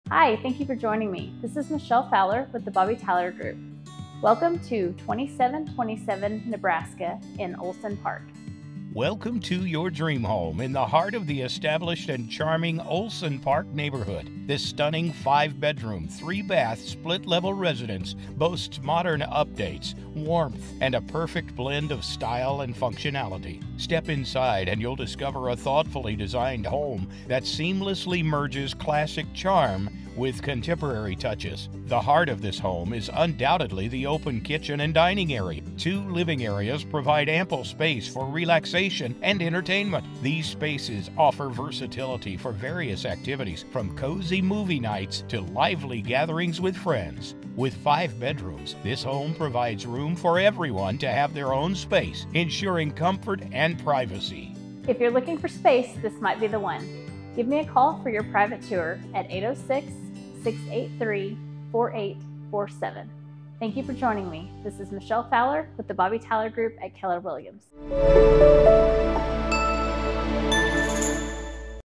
For Sale, Olsen, 2727 Nebraska, Amarillo, TX by Michelle Fowler
Welcome to your dream home in the heart of the established and charming Olsen Park neighborhood! This stunning 5-bedroom, 3-bathroom split-level residence boasts modern updates, warmth, and a perfect blend of style and functionality. Step inside, and you'll discover a thoughtfully designed home that seamlessly merges classic charm with contemporary touches. The heart of this home is undoubtedly the open kitchen and dining area. The kitchen is a chef's delight, featuring modern subway tile, and butcher block countertops that provide both style and functionality. Two living areas provide ample space for relaxation and entertainment. These spaces offer versatility for various activities, from cozy movie nights to lively gatherings with friends. With five bedrooms, this home provides room for everyone to have their own space, ensuring comfort and privacy.